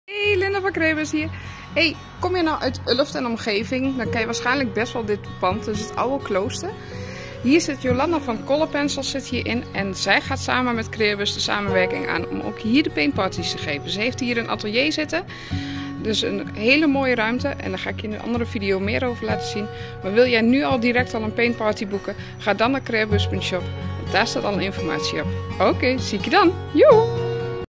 Nieuwe locatie voor paintparty's in Ulft bij ColorPencils
Super leuk, Jolanda van ColorPencils gaat met CreaBus de samenwerking aan en kunnen er nu ook in Ulft creatieve feestjes gegeven worden.
Workshop schilderen voor kinderen en volwassen.